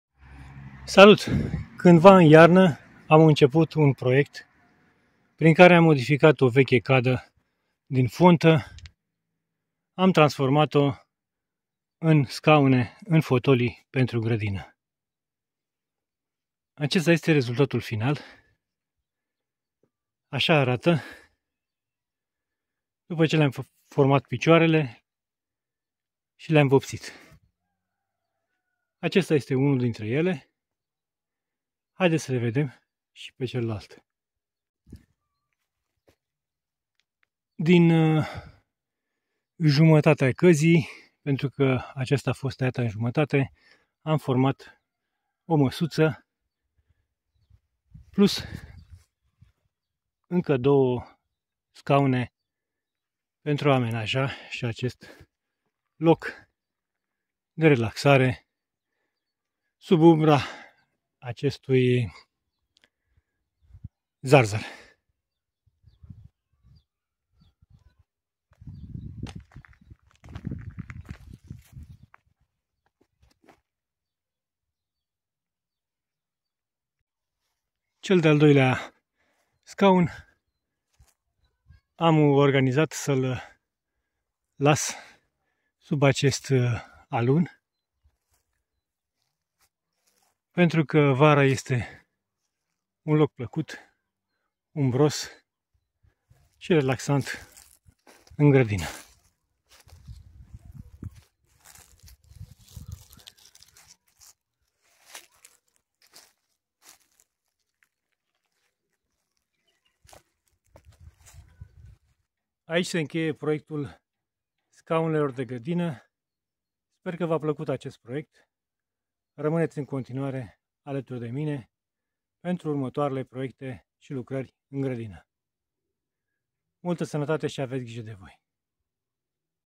Final de proiect -scaune de grădină dintr-o cadă veche.
Dintr-o cada veche am confectionat doua fotolii de gradina si o masuta.